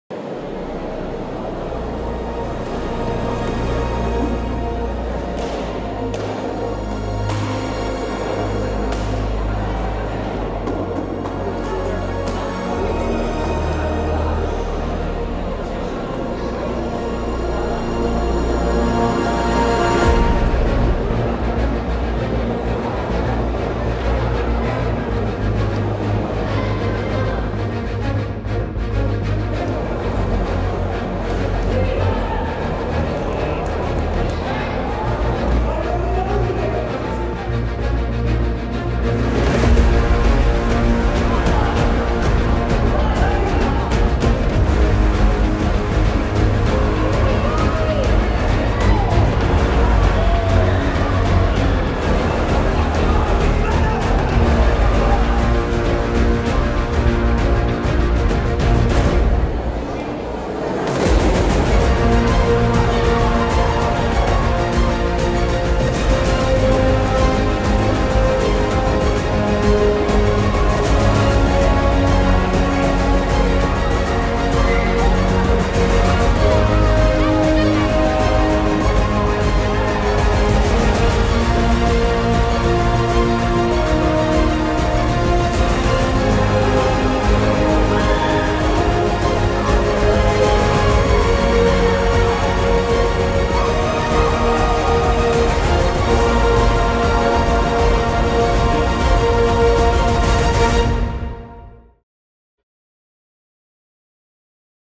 Spot Tae Kwon Do - FITA-CONI
spot promozionale sportivo Tae Kwon Do - Federazione ufficiale CONI ITALIA.

Produttore - CONI-FITA Pesaro
Regia - Federico Lucarini
Operatori - Federico Lucarini
Montaggio & post produzione - Federico Lucarini